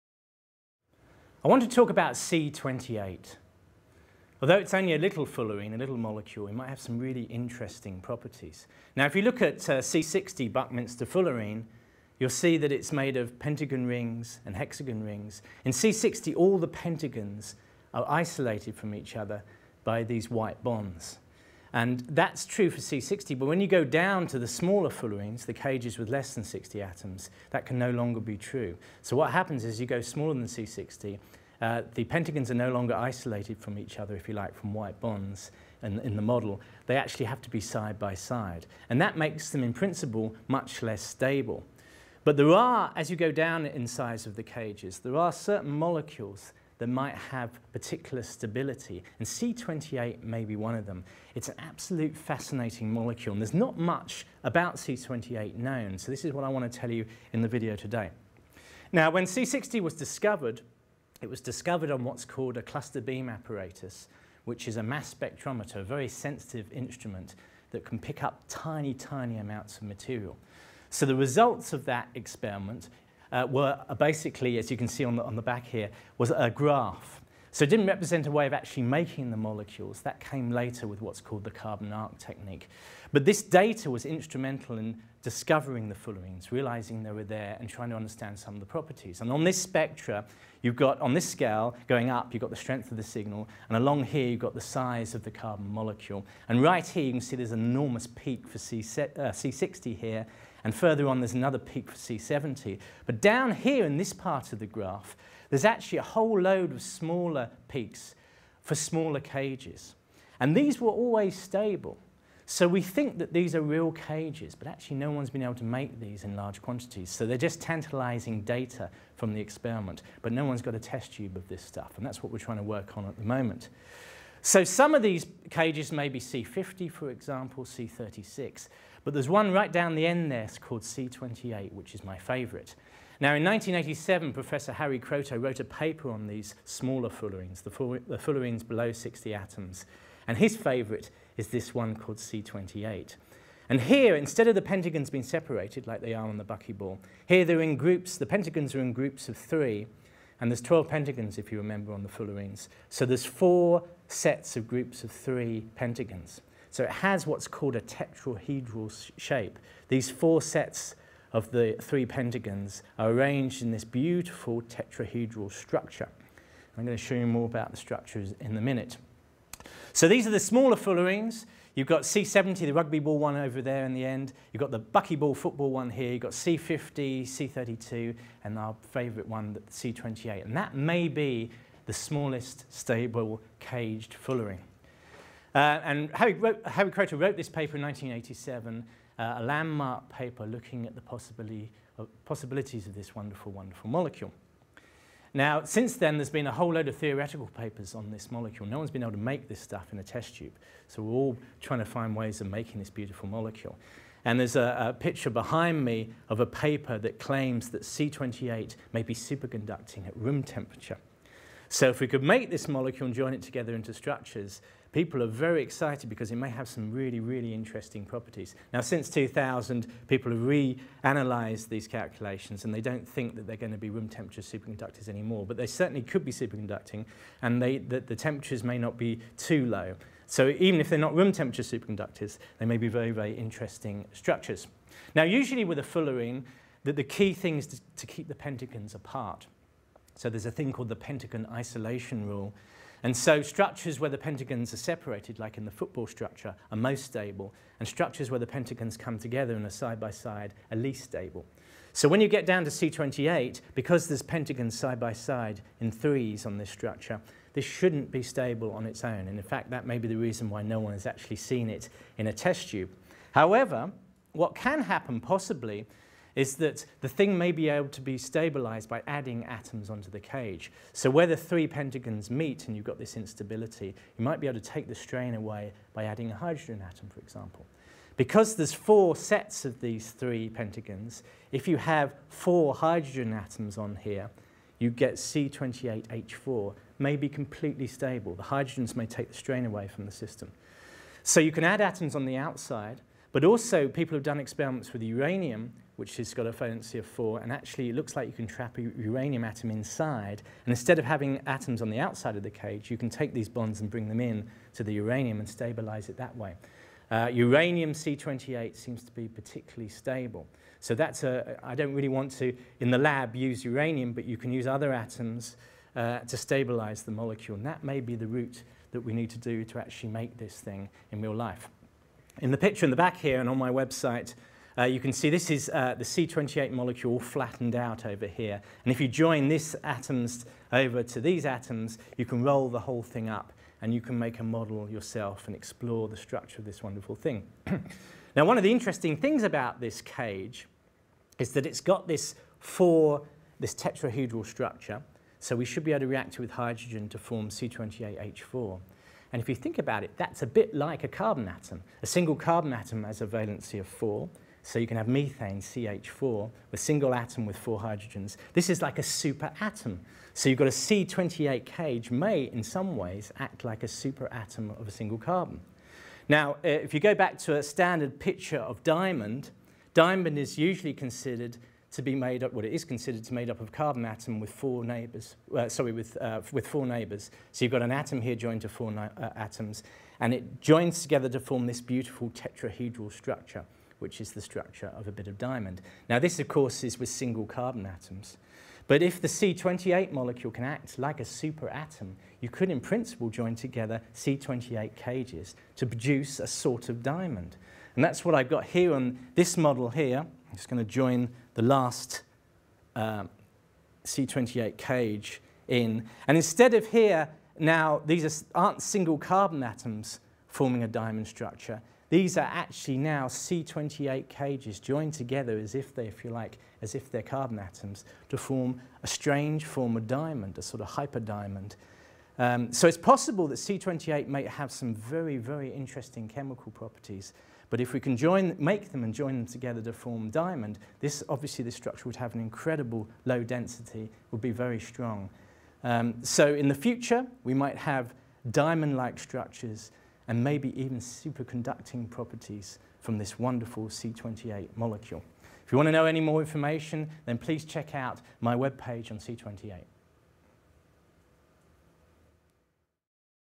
The Carbon Revolution Part 8 C28
The Carbon Revolution Part 8 - Fullerene C28 with Dr Jonathan Hare. The small Fullerenes below C60 were discovered in 1985 but still haven't been made in test-tube quantities. C28 looks to be a most interesting structure. This is an introduction to C28. Keywords: C28, small fullerenes, C60. Filmed 2013